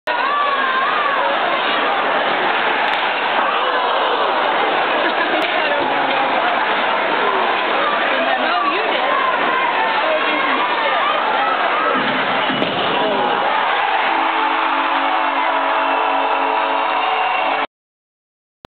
Jan31 Waterloo 2nd Goal
Black Hawks vs. Chicago -- 2nd goal scored by  Jordan Samuels-Thomas  with the assist from Lee Moffie and  Craig Smith at 15:44 in the 2nd period.